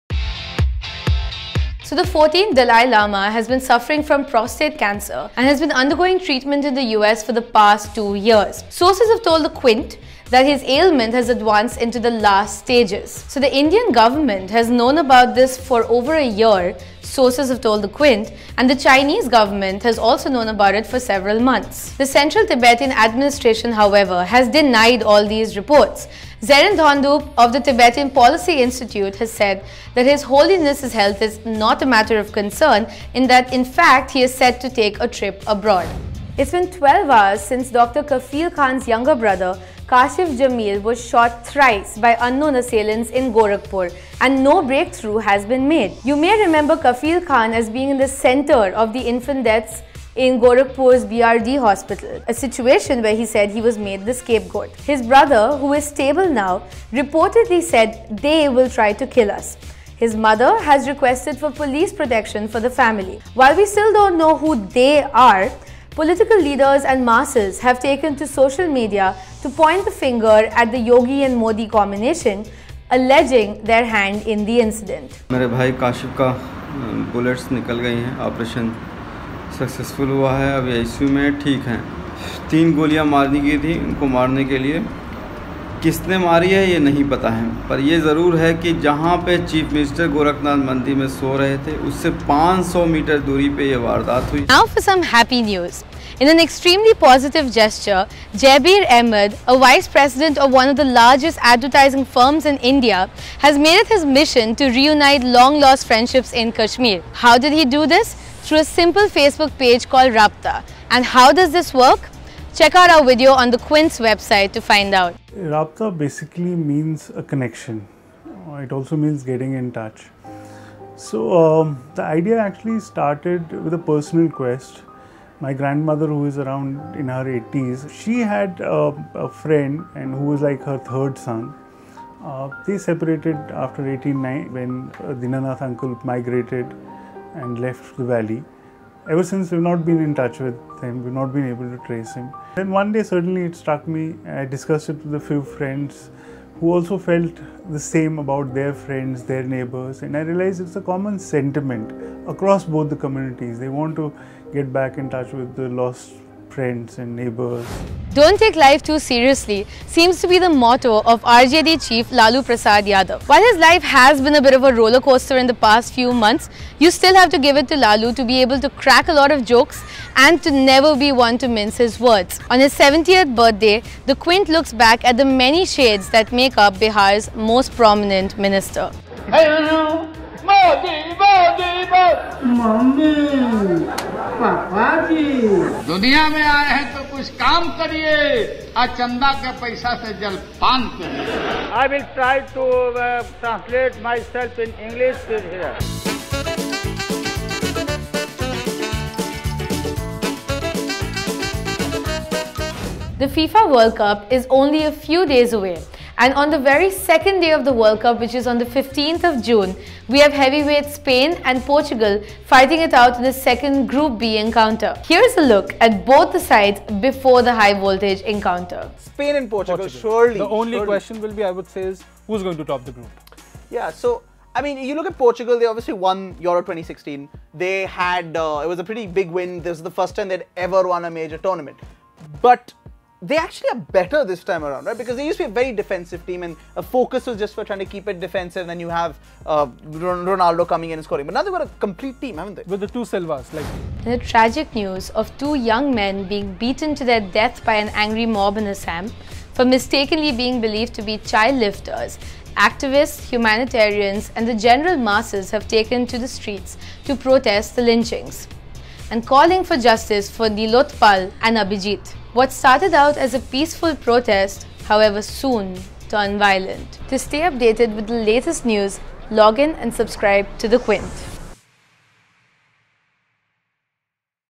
QWrap: Dalai Lama Terminally Ill; Dr Kafeel Khan’s Brother Shot At
From Dalai Lama suffering from prostate cancer to Kafeel Khan’s brother shot thrice, here are the top stories for the day.